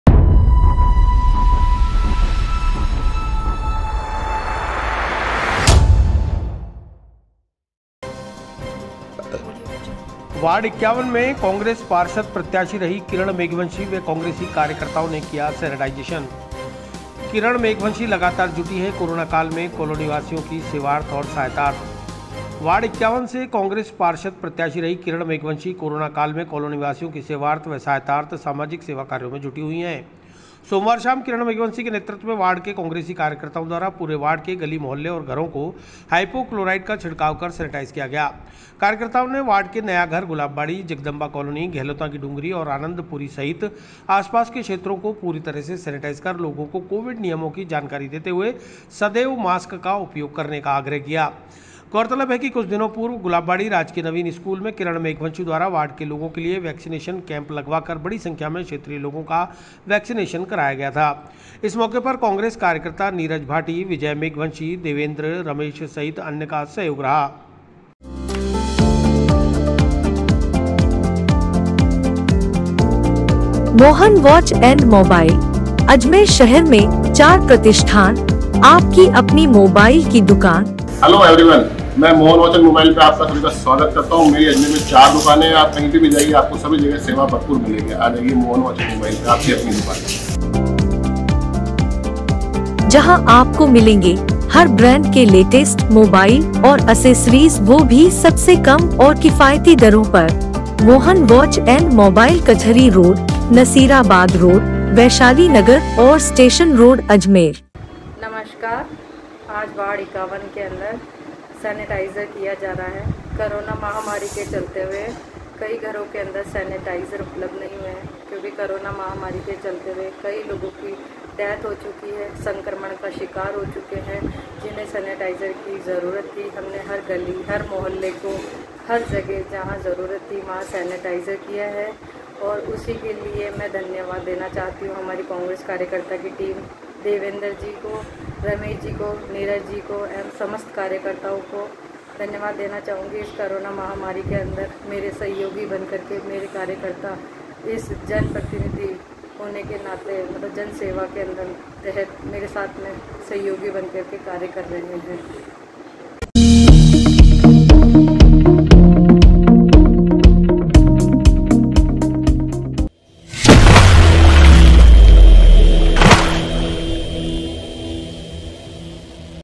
वार्ड 51 में कांग्रेस पार्षद प्रत्याशी रही किरण मेघवंशी व कांग्रेसी कार्यकर्ताओं ने किया सेन्टाइजेशन
किरण मेघवंशी लगातार जुटी हैं कोरोना काल में कॉलोनी वासियों की सेवार्थ और सहायतार्थ

वार्ड 51 से कांग्रेस पार्षद प्रत्याशी रही किरण मेघवंशी कोरोना काल में कॉलोनी वासियों की सेवार्थ व सहायतार्थ सामाजिक सेवा कार्यो में जुटी हुई है।
 सोमवार शाम किरण मेघवंशी के नेतृत्व में वार्ड के कांग्रेसी कार्यकर्ताओं द्वारा पूरे वार्ड के गली, मोहल्ले और घरों को हाइपोक्लोराइट का छिड़काव कर सेनेटाइज किया गया।
कार्यकर्ताओं ने वार्ड के नया घर गुलाब बाड़ी,जगदंबा कॉलोनी, गहलोता की डुंगरी ओर आनंद पुरी सहित आसपास के क्षेत्रों को पूरी तरह सेनेटाइज कर लोगों को कोविड नियमों की जानकारी देते हुए सदैव मास्क का उपयोग करने का आग्रह किया।
गौरतलब है कि कुछ दिनों पूर्व ग़ुलाबबाड़ी राजकीय नवीन स्कूल में किरन मेघवंशी द्वारा वार्ड के लोगों के लिए वेक्सिनेशन कैम्प लगवा कर बड़ी संख्या में क्षेत्रीय लोगों का वेक्सीनेशन कराया था। इस मौके पर कांग्रेस कार्यकर्ता नीरज भाटी, विजय मेघवंशी, देवेंद्र रमेश सहित अन्य का सहयोग रहा।